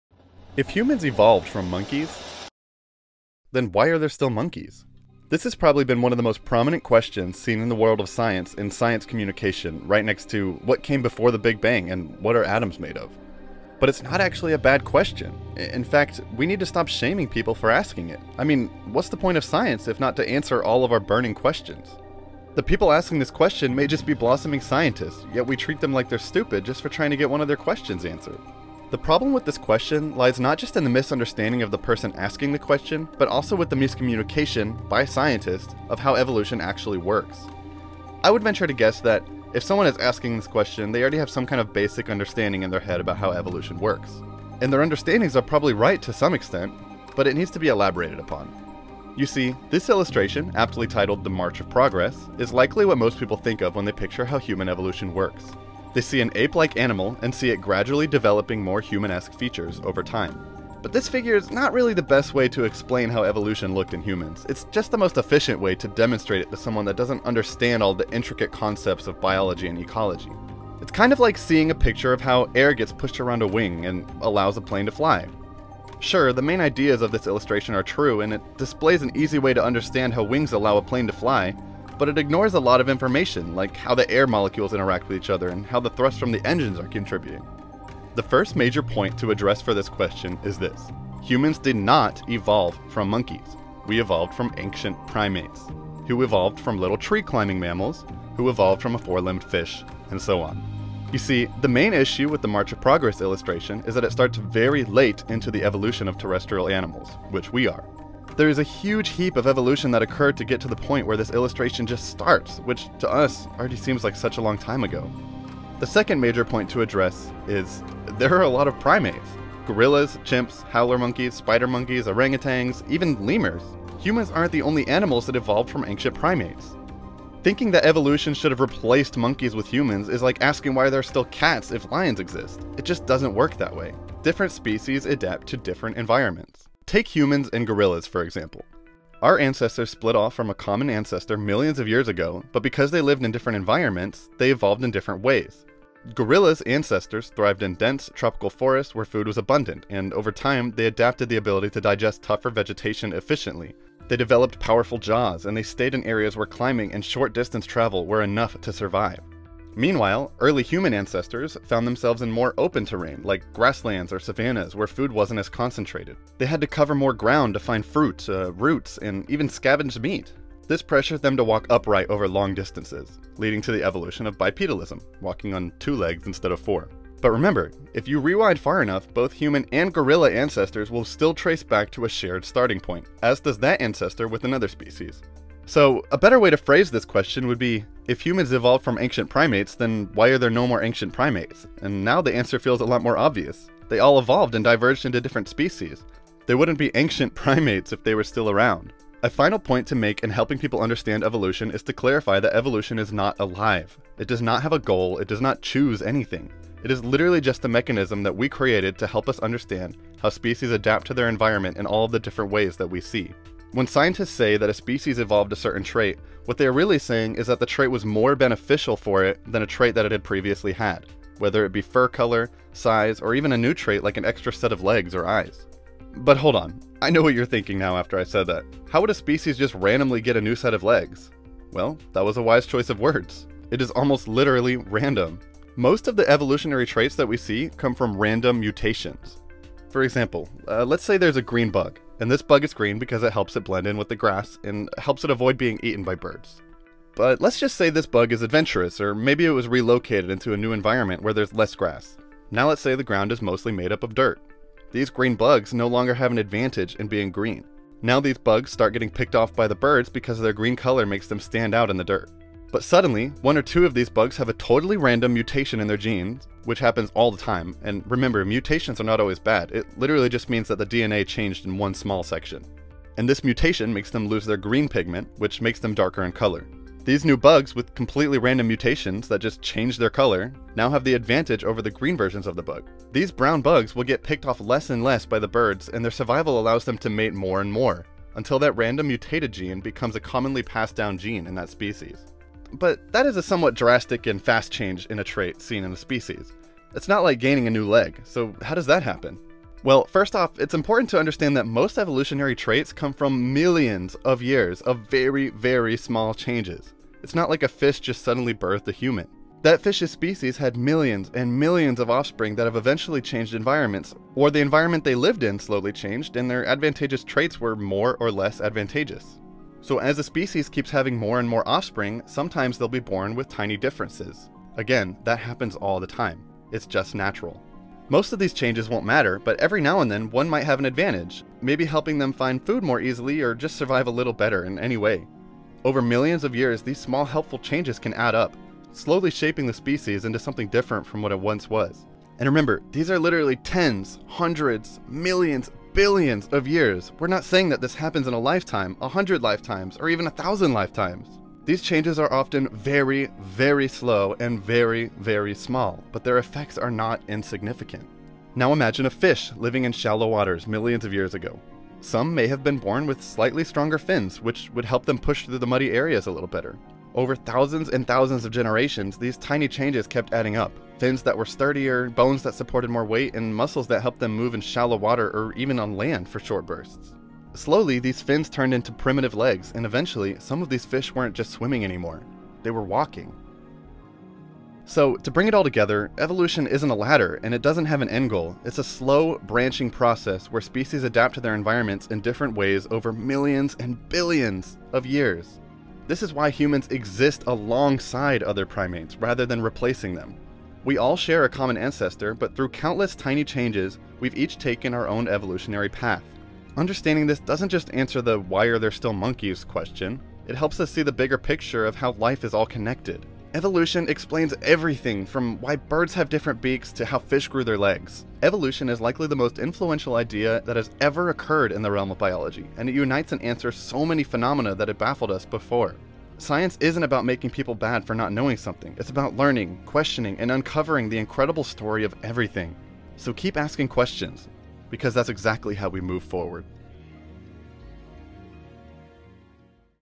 If Humans Evolved From Monkeys...
If Humans Evolved from Monkeys, Why Are There Still Monkeys?

Great question! This video should cover everything you need to know to understand the answer to this question, without getting into the nitty gritty of the science of it all. Even though it’s awesome, and you should totally look deeper into it!

Science helps us understand big questions like this and see the world in a whole new way. The more we learn, the more amazing everything becomes!